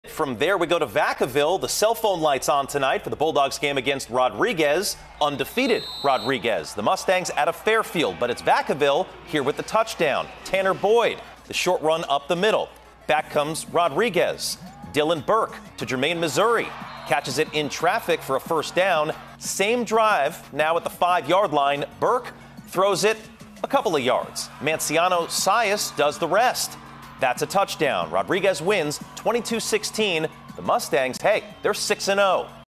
Rodriguez def Vacaville, 22-16
The Mustangs from Fairfield improve to 6-0 this season with a 22-16 win at Vacaville on Friday night.